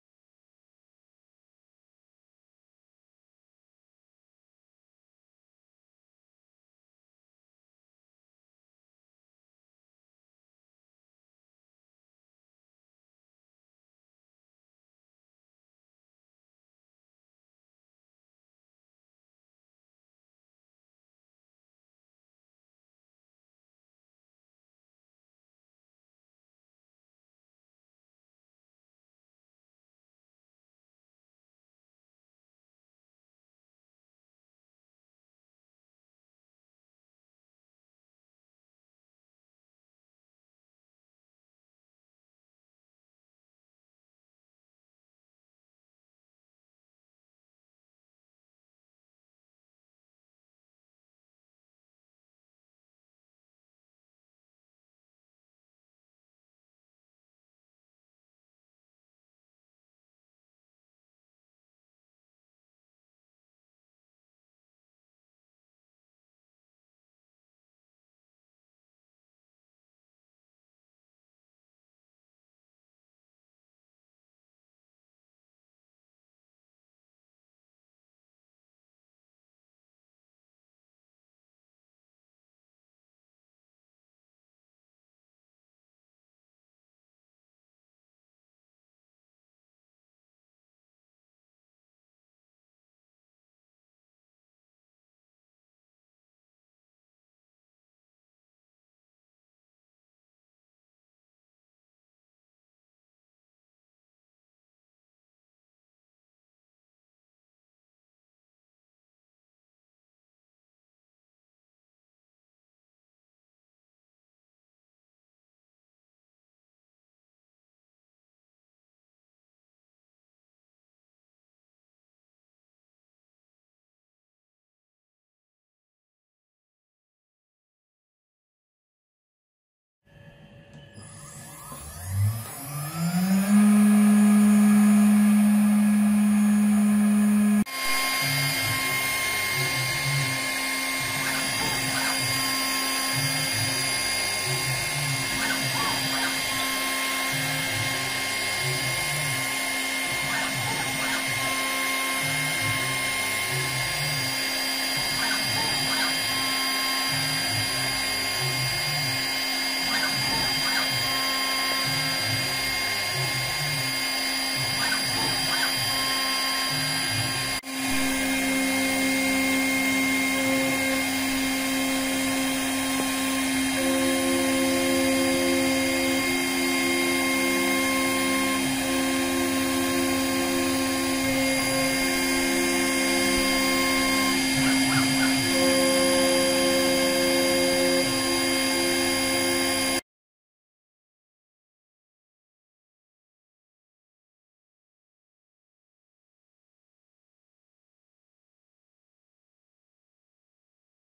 Fresadora económica modelo 6040E
Fresadora CNC , modelo 6040E, serie económica, con grandes prestaciones, husillos a bolas, patines lineales a bolas, disponibles opciones con control, base ranurada etc. correo para consultas